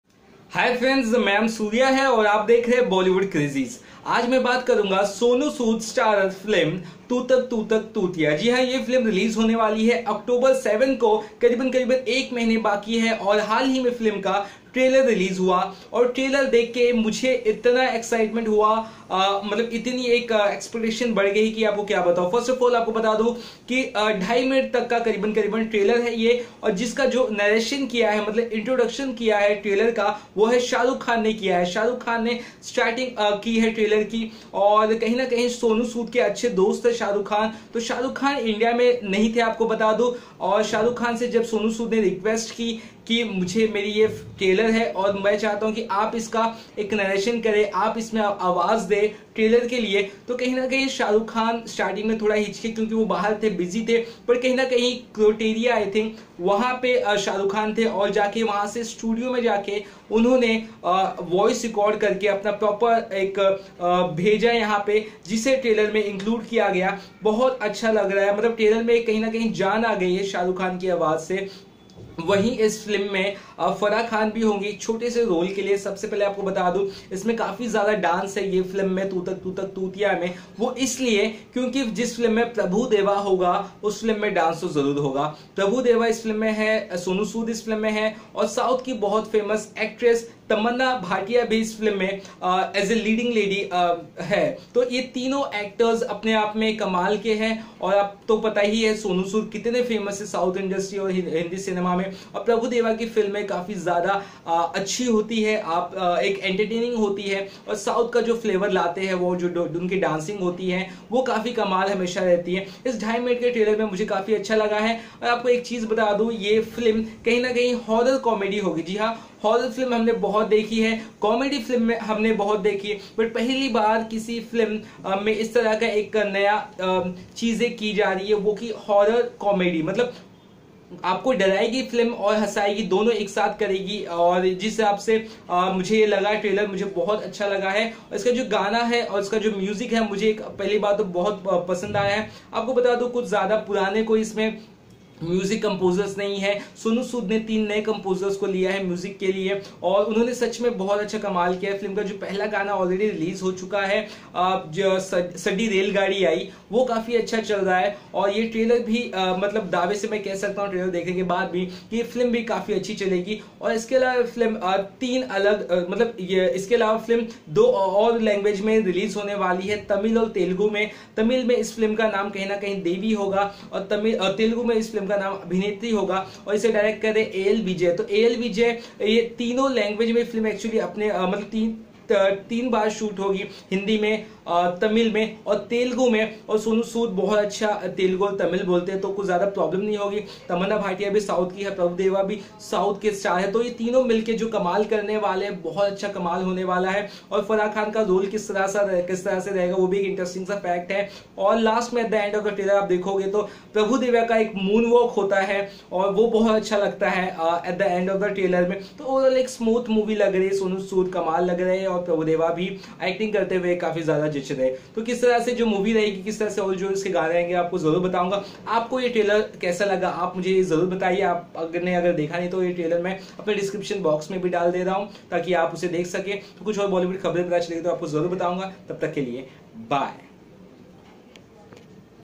Tutak Tutak Tutiya Trailer Review
Sonu Sood, Prabhudeva and Tamannaah are all set to entertain us in the upcoming flick Tutak Tutak Tutiya. At latest, the trailer of the film has been released and I must say, it's a good trailer.

Watch Out The Trailer Right Here: Tutak Tutak Tutiya